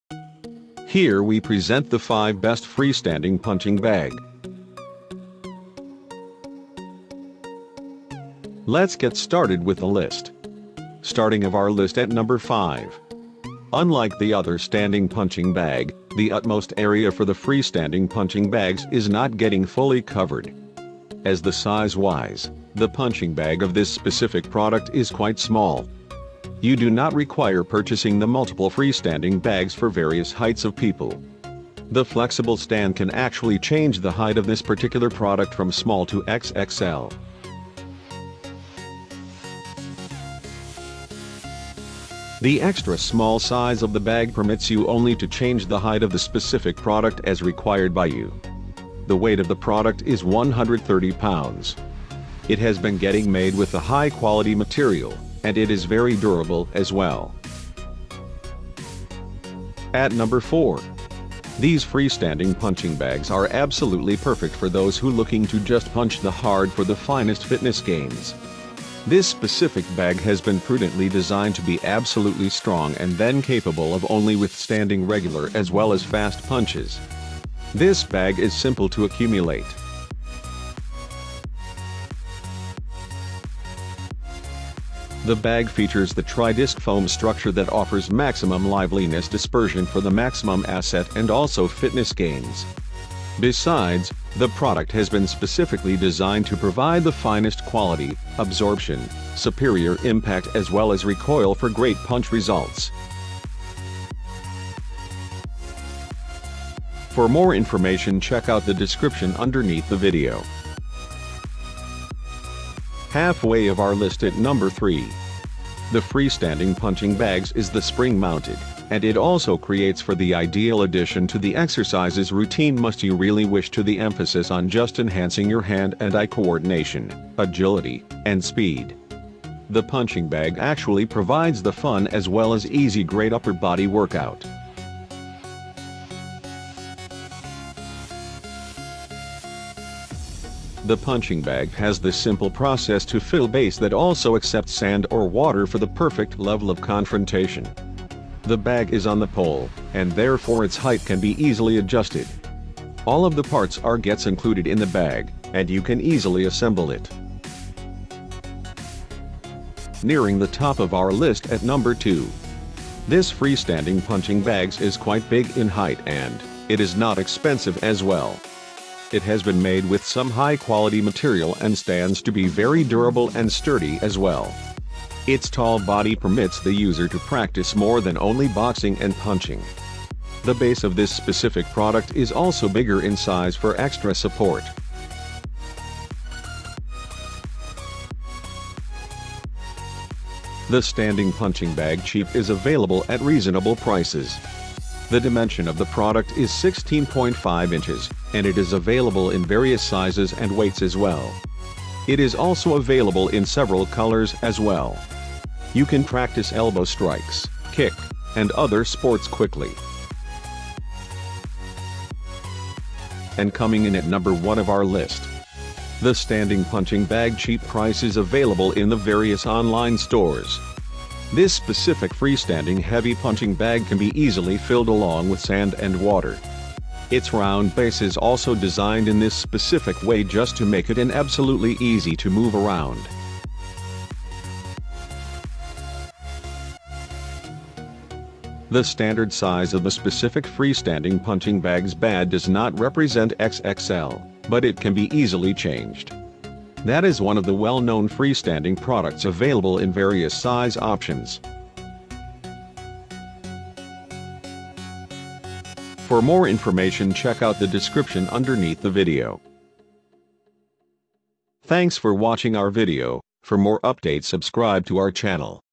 Best Free Standing Punching Bag
Are you looking for the Best Free Standing Punching Bag. We spent hours to find out the Best Sleeping Bag for you and create a review video. In this video review you will find the top products list, what is the feature of these products and why you should buy it.

Here is the list of 5 Best Free Standing Punching Bag

1. Costzon 2 FT Kids Punch Bag

2. Velocity Boxing Children's Pro

3. Hyperflex Strike Bag

4.Everlast 2228 PowerCore

5. Everlast Omniflex Free